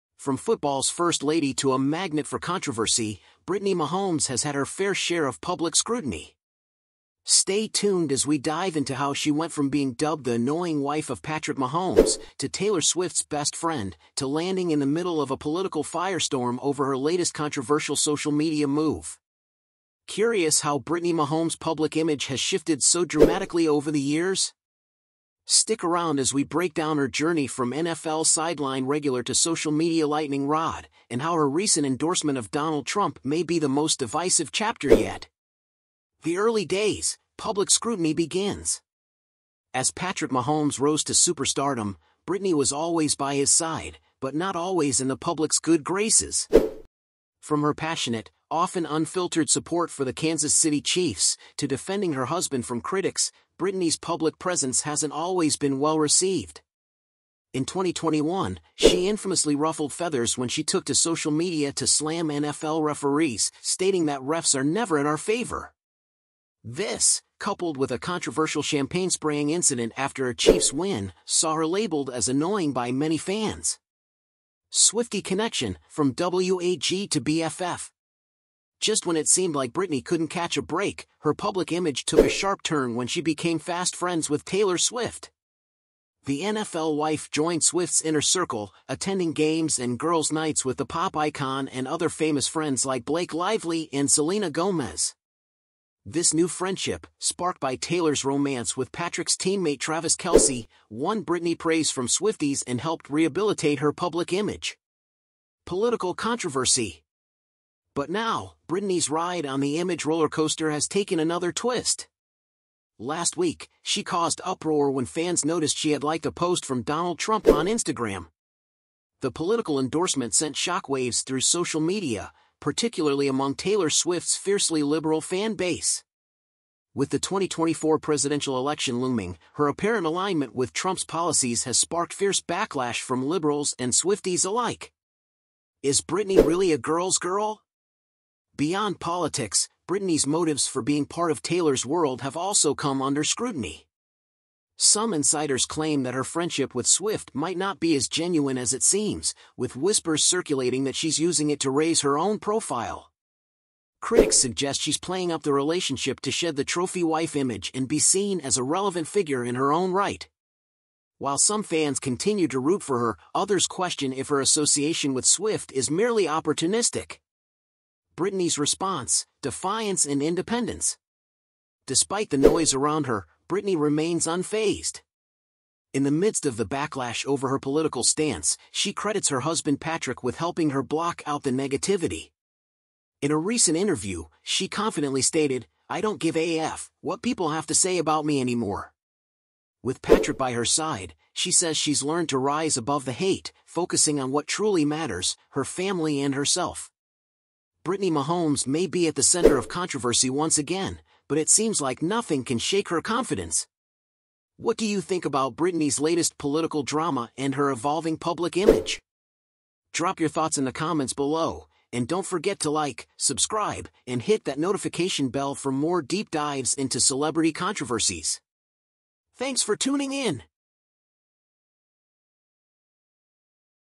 Brittany Mahomes: From 'Annoying' Wife to Swift's Bestie Amid Trump Controversy
From football's first lady to a magnet for controversy, Brittany Mahomes has had her fair share of public scrutiny. Stay tuned as we dive into how she went from being dubbed the "annoying" wife of Patrick Mahomes, to Taylor Swift's best friend, to landing in the middle of a political firestorm over her latest controversial social media move.